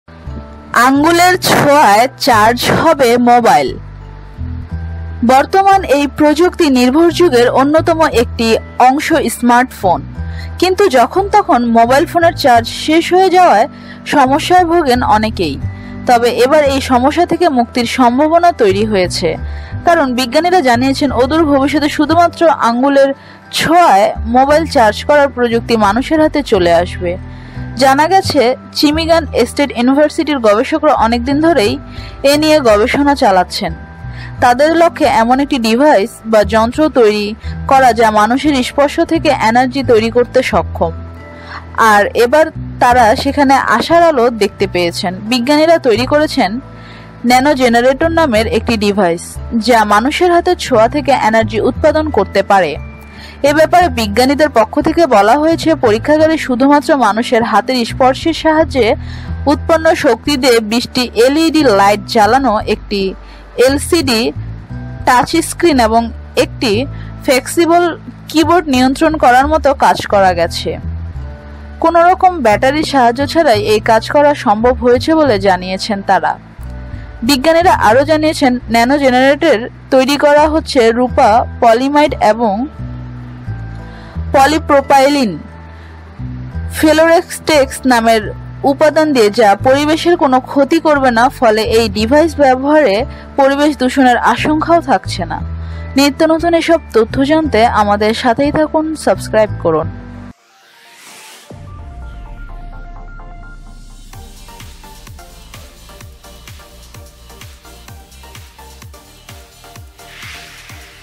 আঙুলের ছোঁয়ায় চার্জ হবে মোবাইল
মিসিগান স্টেট ইউনিভার্সিটির গবেষকরা অনেকদিন ধরেই এই নিয়ে গবেষণা চালাচ্ছেন। তাদের লক্ষ্যে এমন একটি ডিভাইস বা যন্ত্র তৈরি করা যা মানুষের স্পর্শ থেকে এনার্জি তৈরি করতে সক্ষম। আর এবার তারা সেখানে আশার আলো দেখতে পেয়েছেন। বিজ্ঞানীরা তৈরি করেছেন ন্যানোজেনারেটর নামের একটি ডিভাইস, যা মানুষের হাতের ছোঁয়া থেকে এনার্জি উৎপাদন করতে পারে।